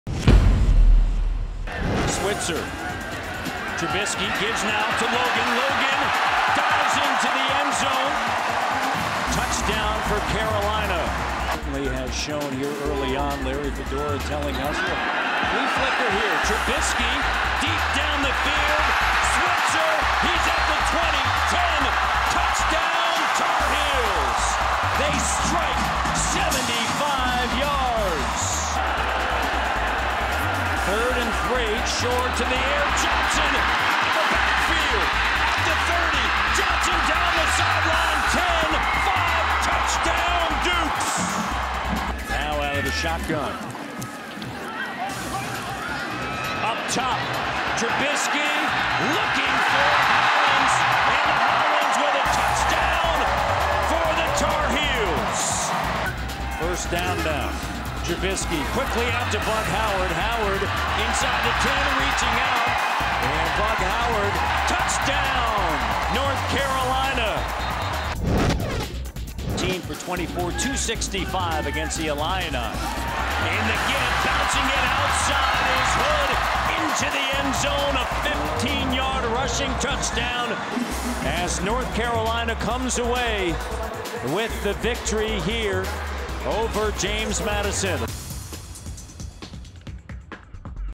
North Carolina vs. James Madison Football Highlights (2016)
North Carolina vs. James Madison: Running backs Elijah Hood and T.J. Logan combined for 5 TDs, and QB Mitch Trubisky threw for a career-high 432 yards to lead the Tar Heels to a 56-28 win. It was a solid tune-up for UNC before its ACC opener next week against Pitt.